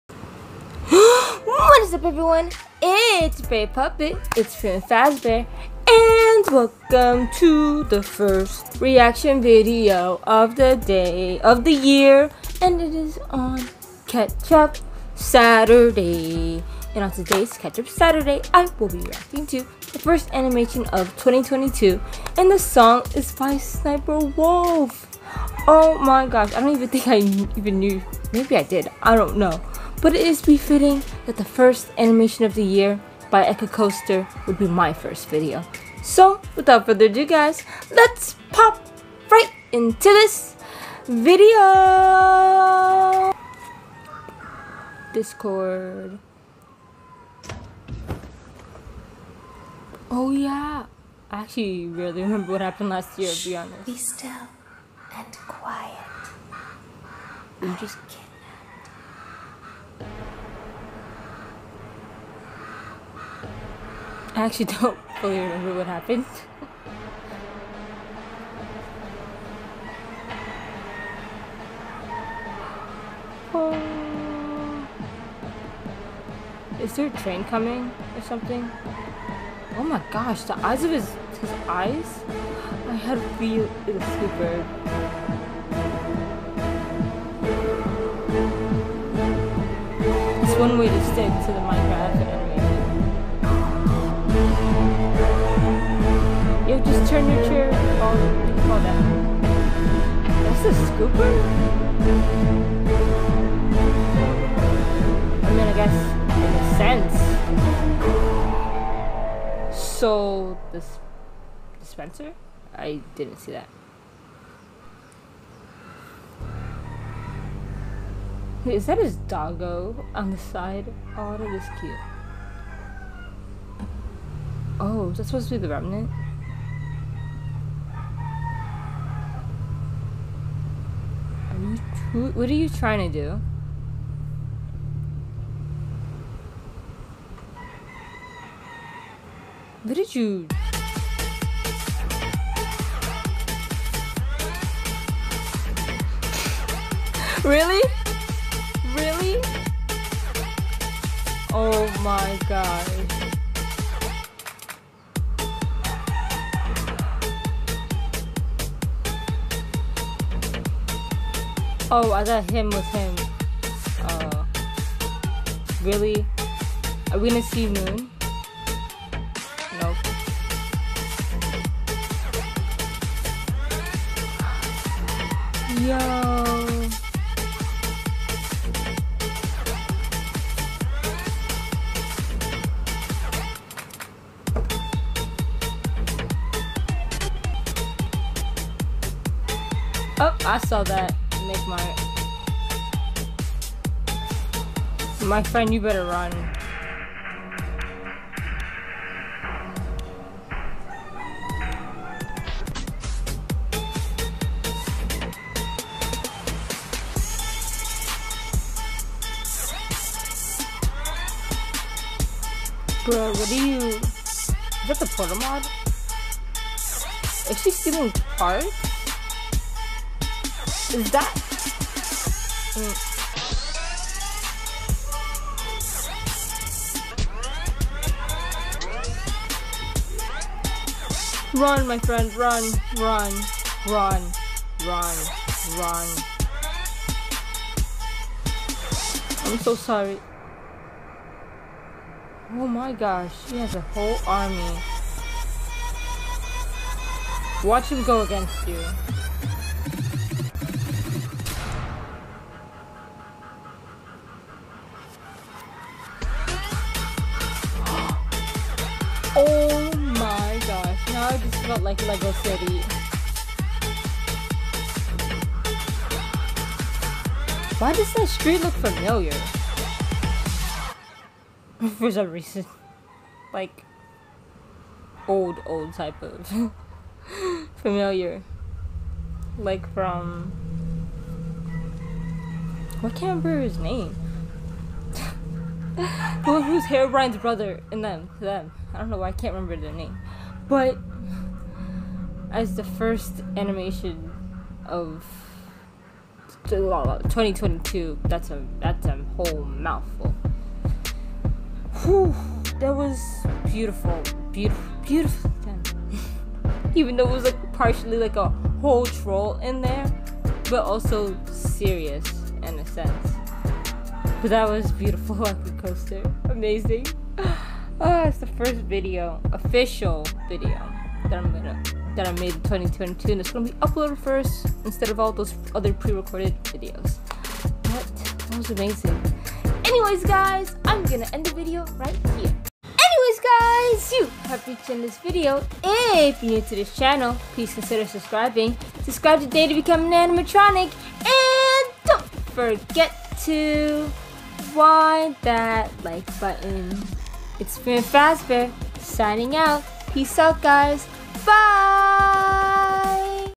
His First Animation, My First Video of 2022 | The FIRST Animation of 2022! (Song by SSSniperWolf)
Hey everyone itz your favorite puppet ItzFemaleMarionette1 Fazbear and welcome back to another reaction video! And today I'm reacting to " The FIRST Animation of 2022! (Song by SSSniperWolf)" AND ANIMATION BY Ekrcoaster.